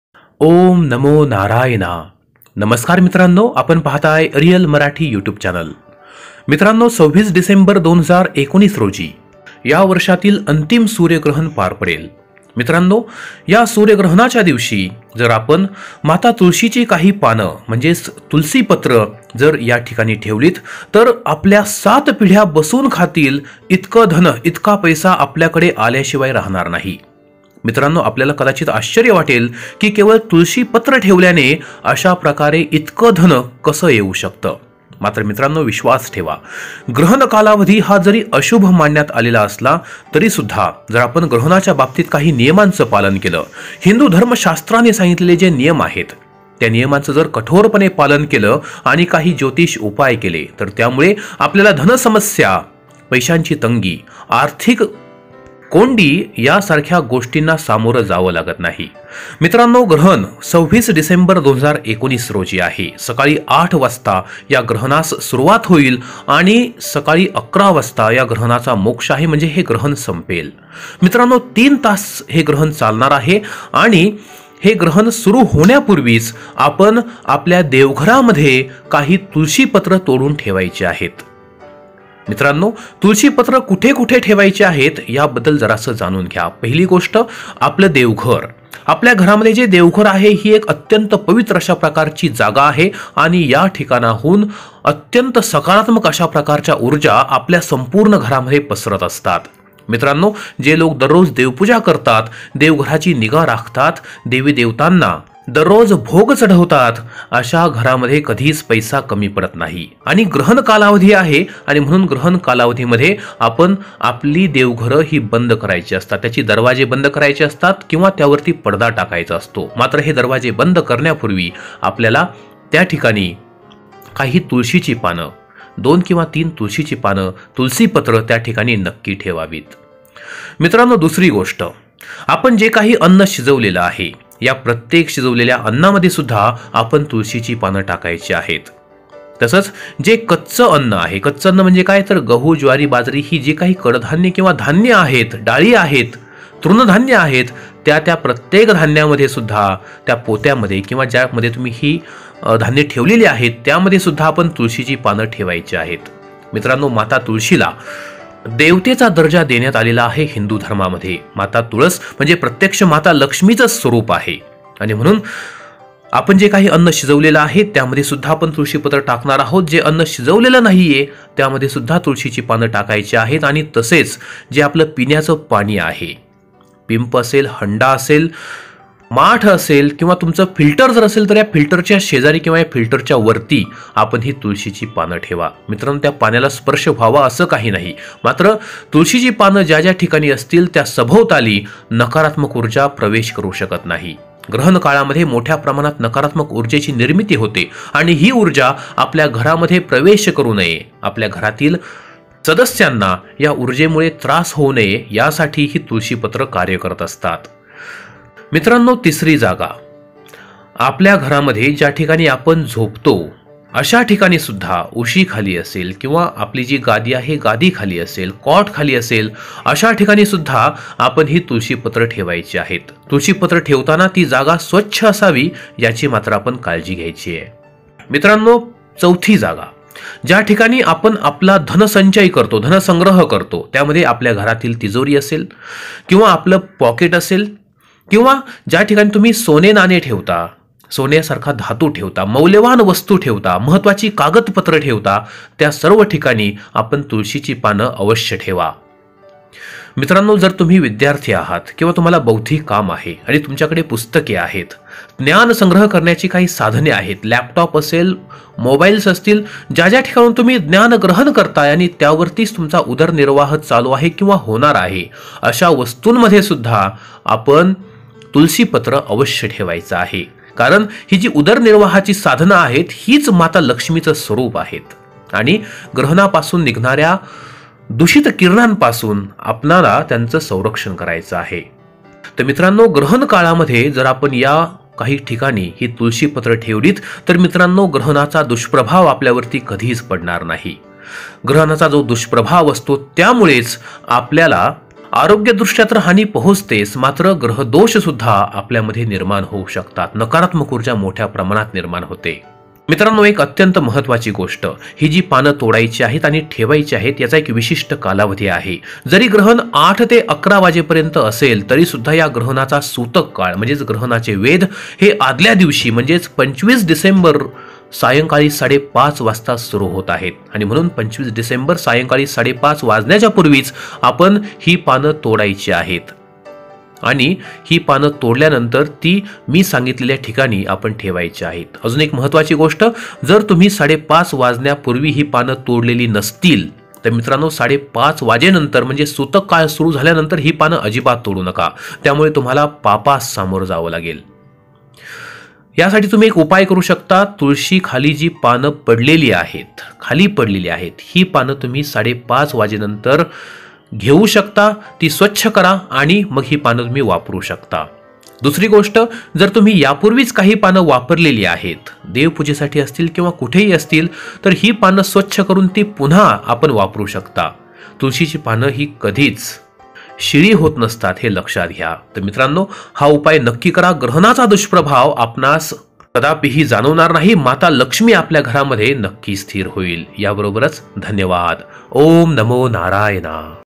26 डिसेंबर 2019 सूर्यग्रहण तुळशीची पाने गुपचूप ठेवा इथे तुमच्या 7 पिढ्या पैशांवर करतील राज्य..
26 डिसेंबर 2019 सूर्यग्रहण तुळशीची पाने गुपचूप ठेवा इथे तुमच्या 7 पिढ्या पैशांवर करतील राज्य..